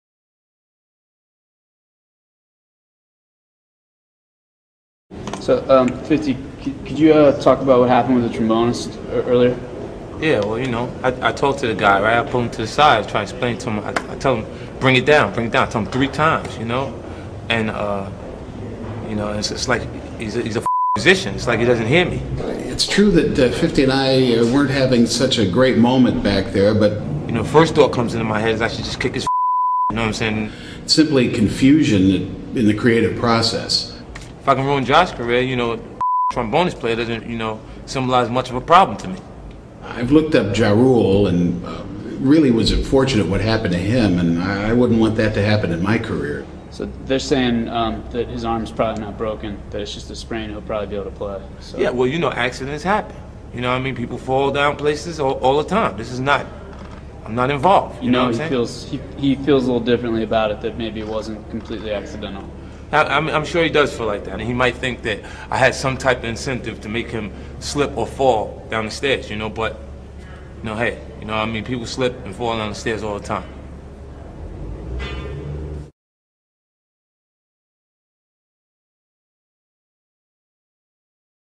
50 Cent - Vitamin Water Outtakes - 50 Disses the Trombonist
Hilarious shit from 50 Cent of G-Unit!